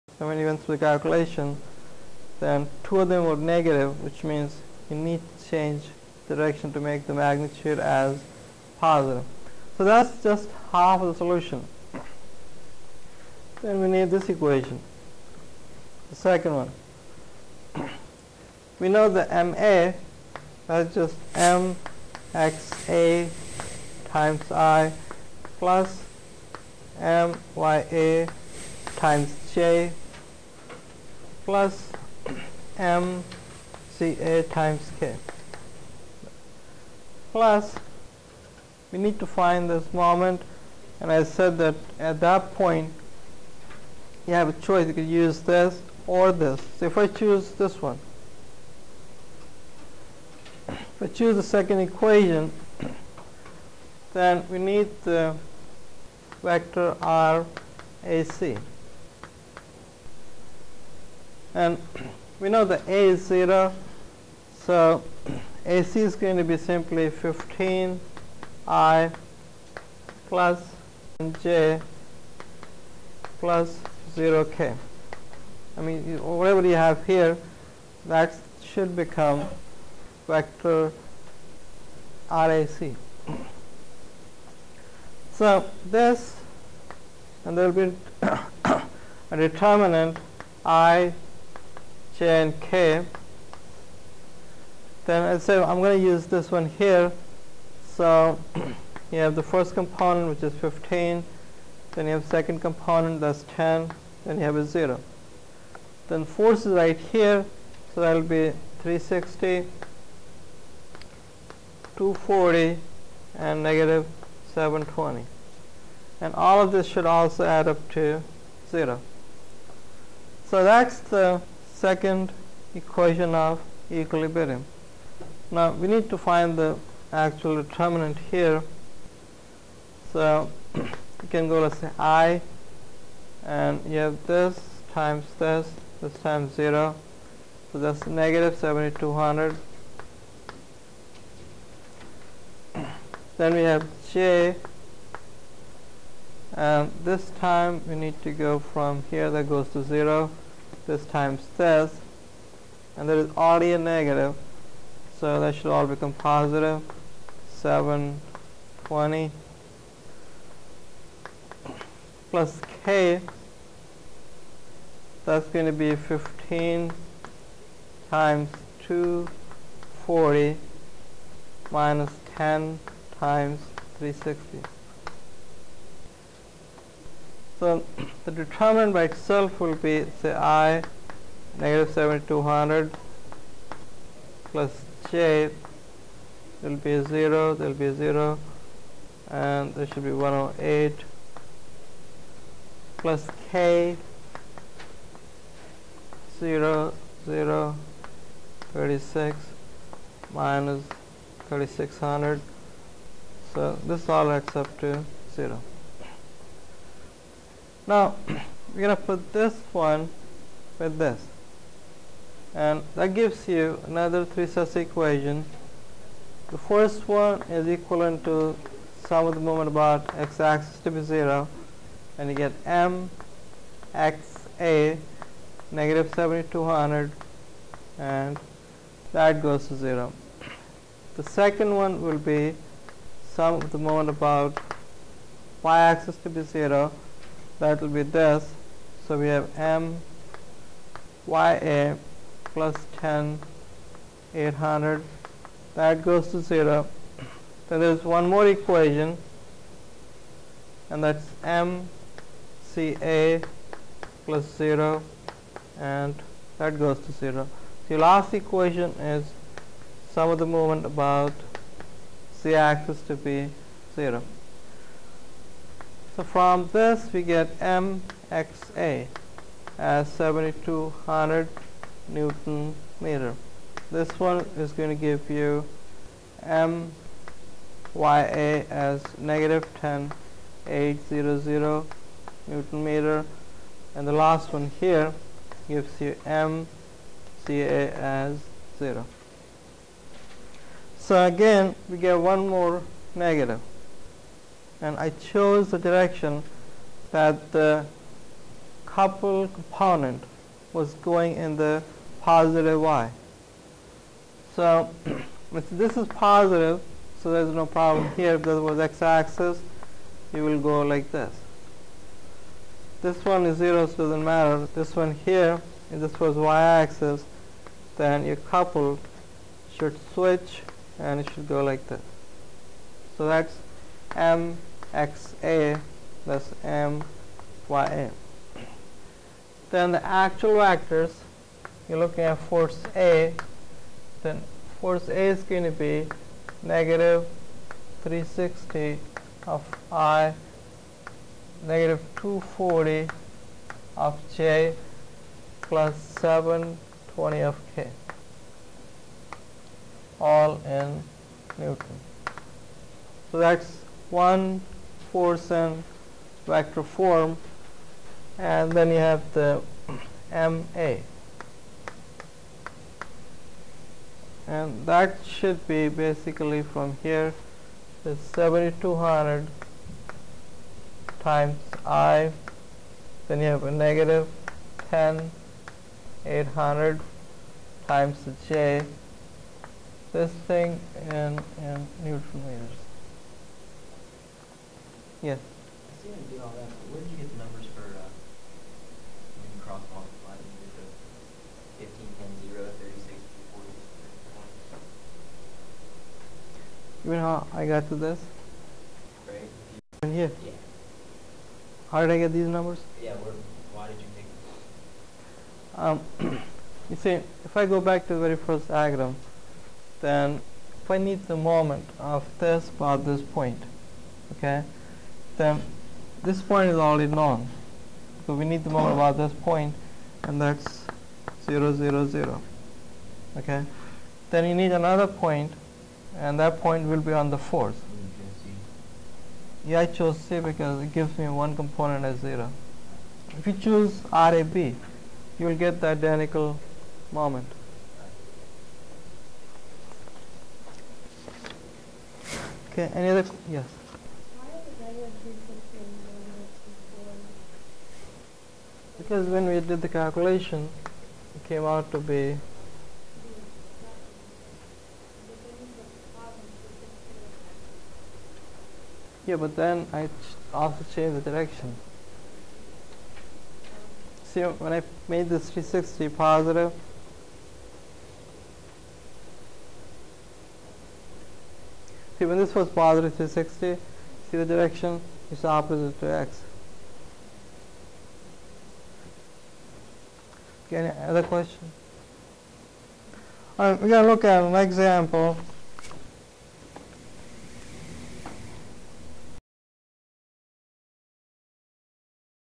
Lecture Example1 3d rigid body equilibrium part C
The video shows the procedure to solve a 3dimensional static equilibrium problem for a rigid body.

Please report any suggestions and problems with the video in the comment section here.